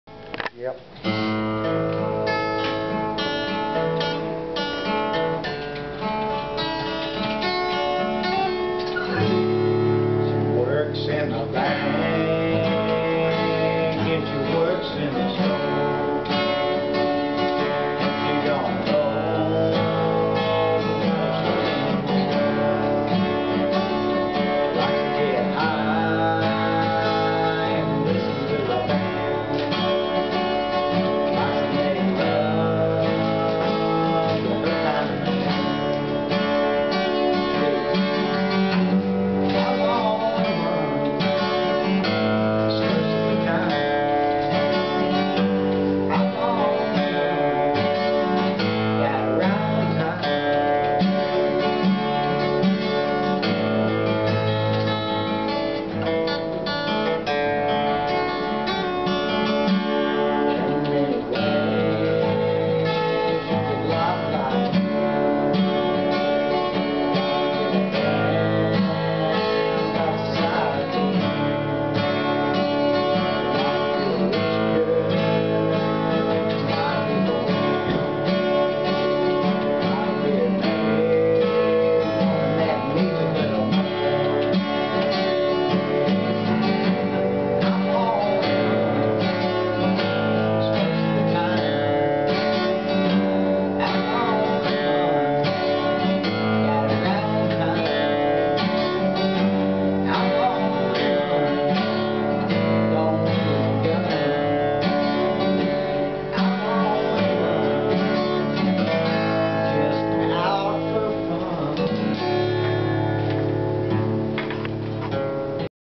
Michael Mitchell and Scotty Gross
Playing in my room at Breathitt High School!  Good job!!  MY SON!!